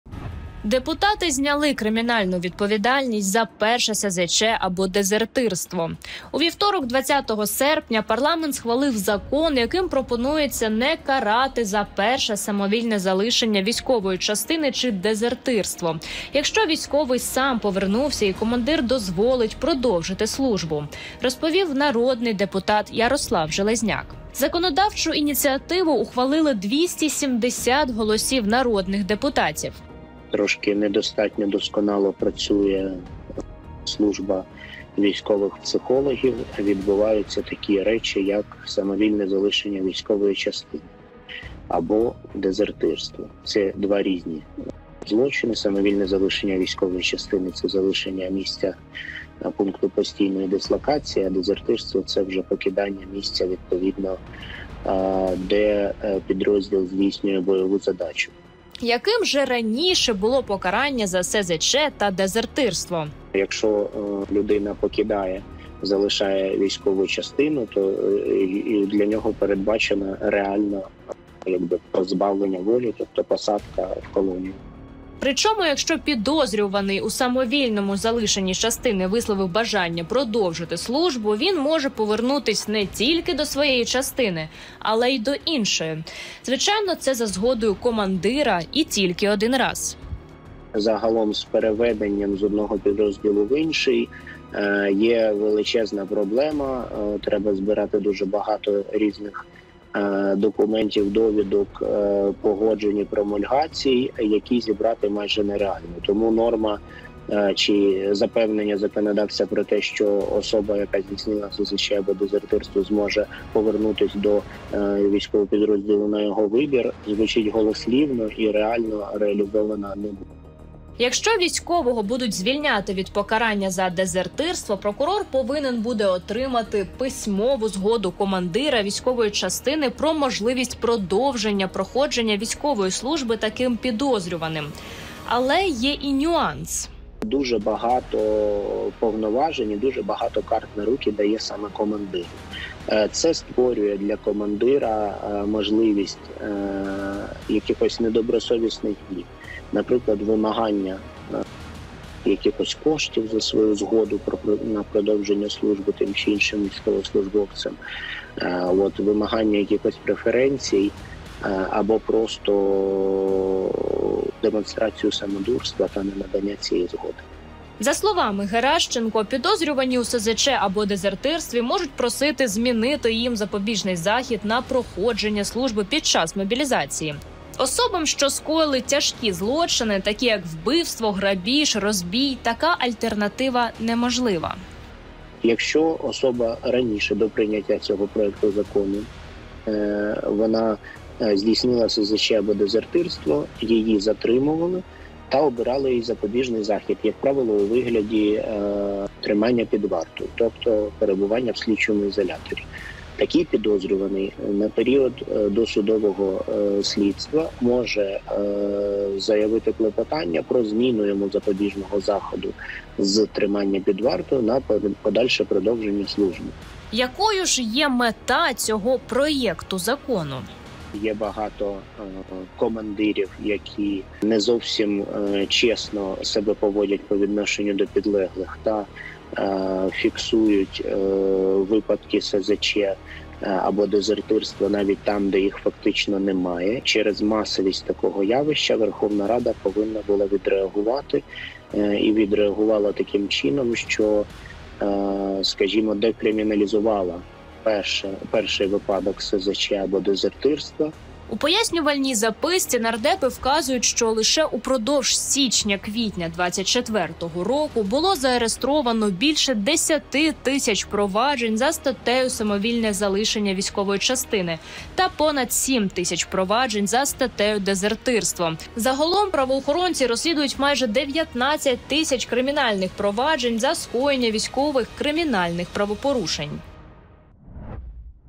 Увага! Прийняли ВАЖЛИВИЙ ЗАКОН в Україні! Яке покарання чекає на українських дезертирів?
Український Парламент схвалив закон, яким пропонується не карати за самовільне залишення військової частини чи дезертирства, якщо військовий сам повернувся і командир дозволив йому продовжити службу. Чи допоможе такий закон виправити ситуацію в Україні – розкажемо в нашому відео.